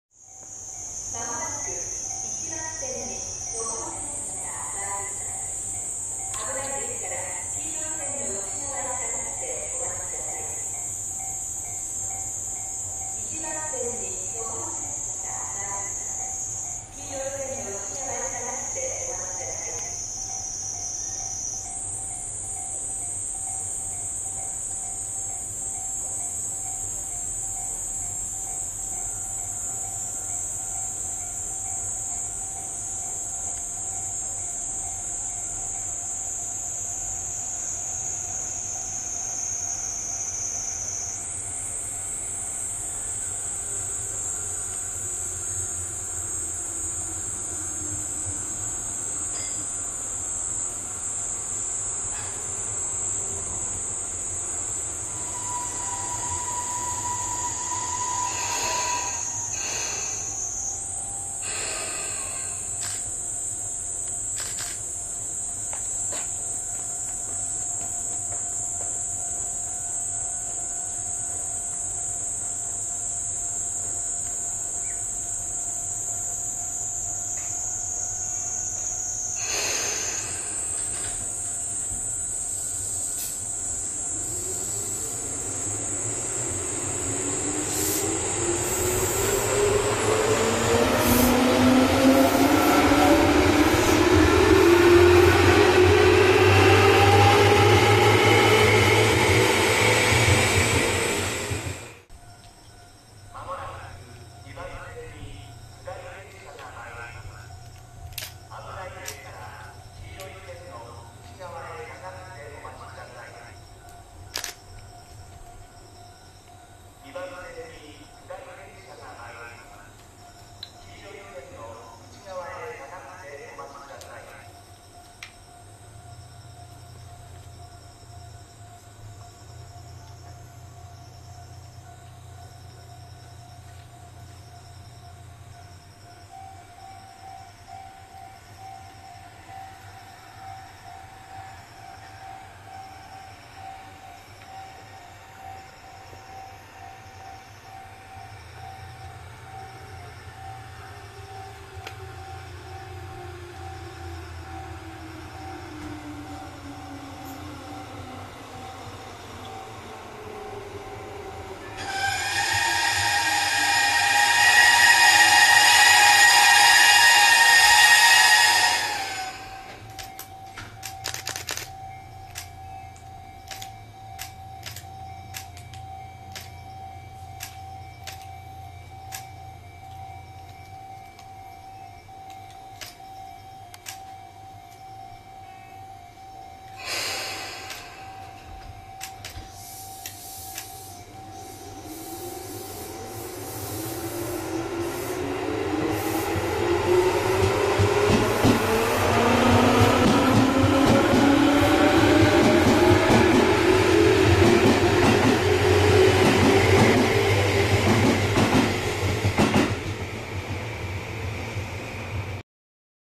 東武10000系10030型11631F 野田線試運転 七里着発
2013/8/4 往復とも七里にて
LCD及び自動放送確認試運転